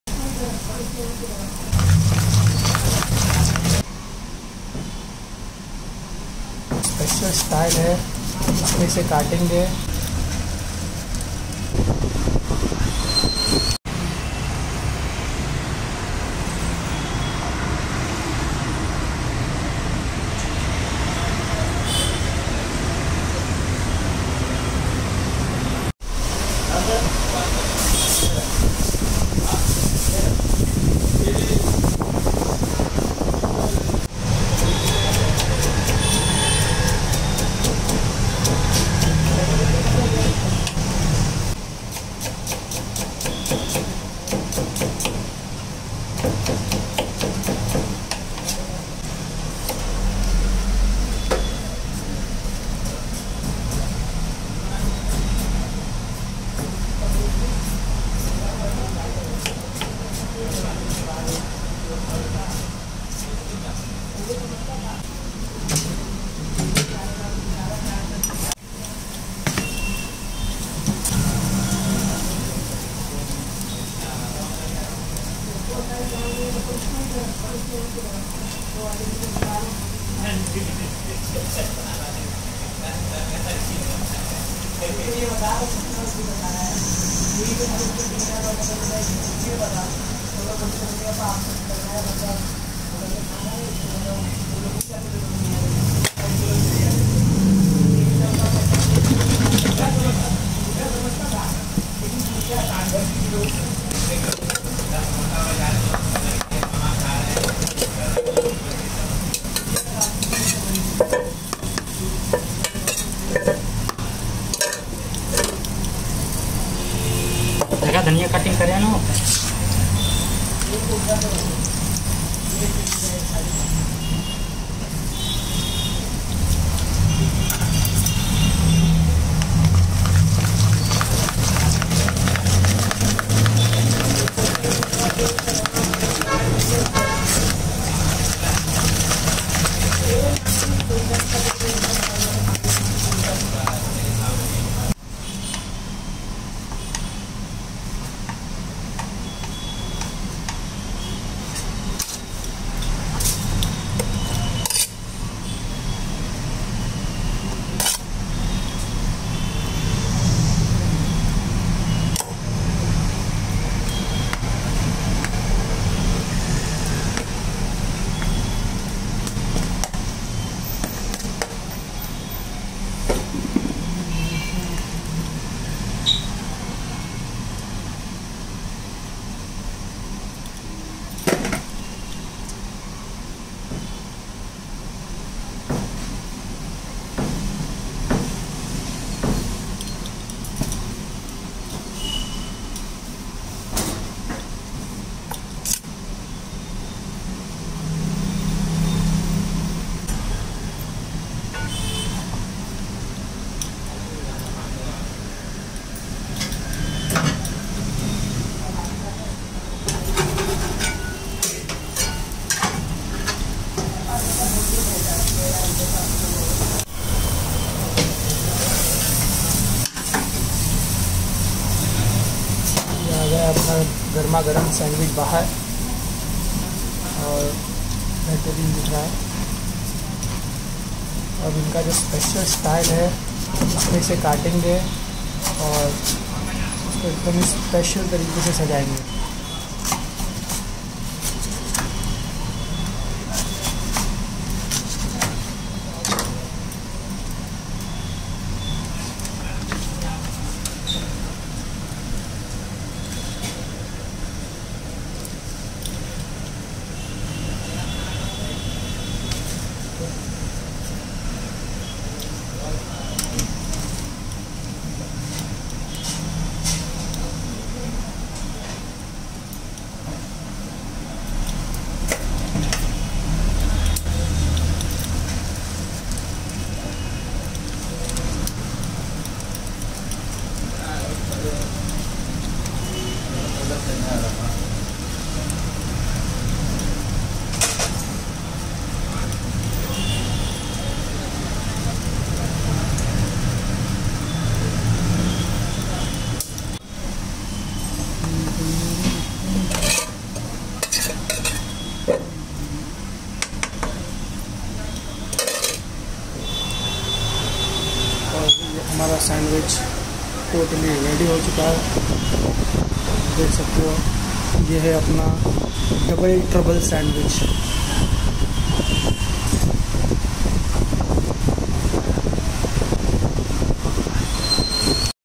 Famous TROUBLE Sandwich of Mumbai 😋 Triple Layer Sandwich | INDIAN STREET FOOD
Enjoy the making of famous Double Trouble Sandwich of Mumbai. Most Unique Sandwich of India. Mixer of Cheese, Tomatoes, Onions and Cabbage. Very innovative idea of presentation.